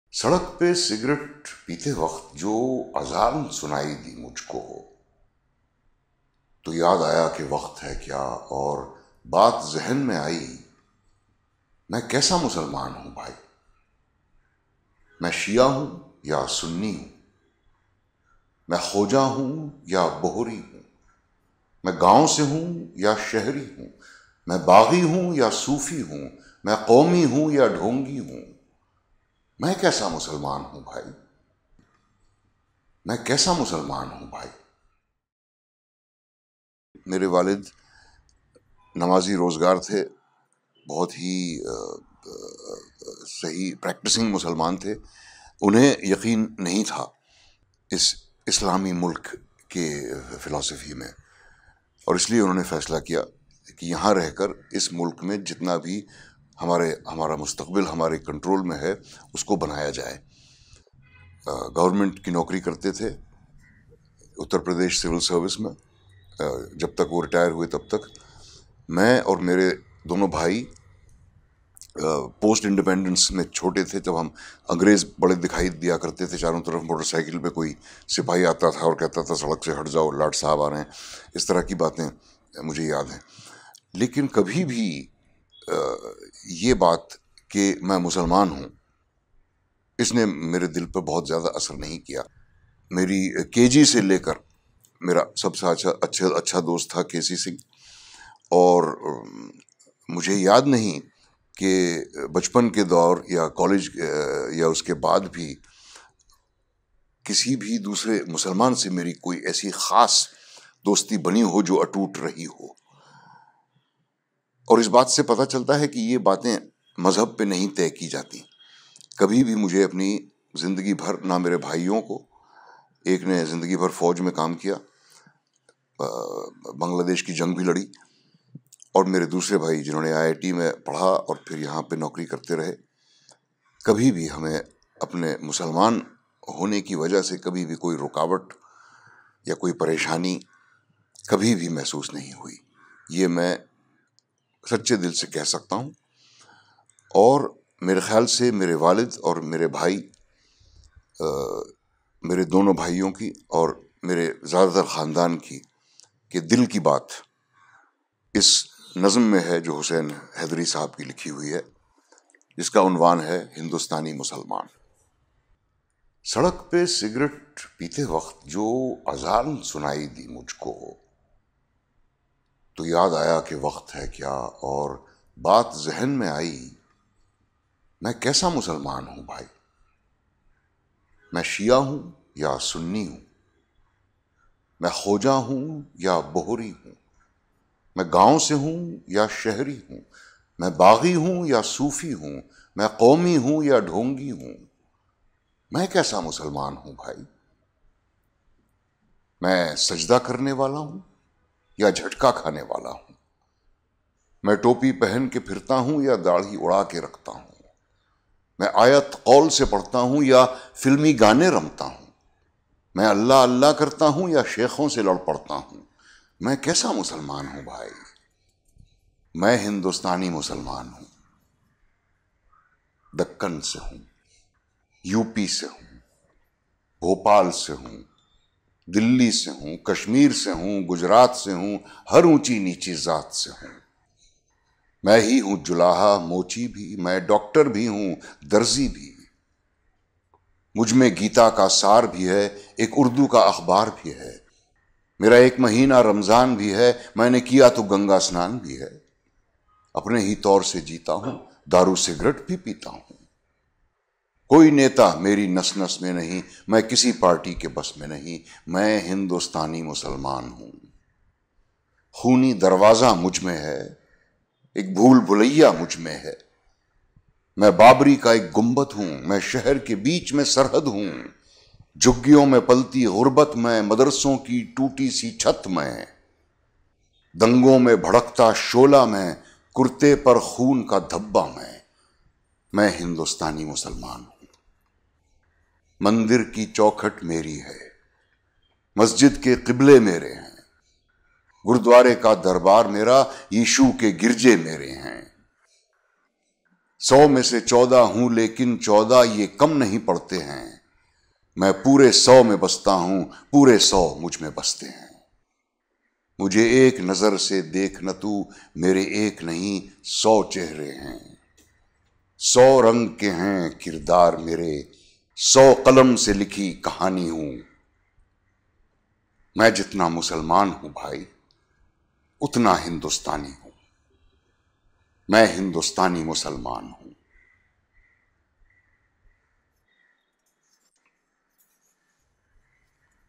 Hindustani Musalman : Hussain Haidry : Naseeruddin Shah in Urdu Studio with Manish Gupta
Created by : Manish Gupta
Camera Editing: Tanu Sharma
Thanks to Ambika Kamal

Reproduction of any kind is prohibited without written permission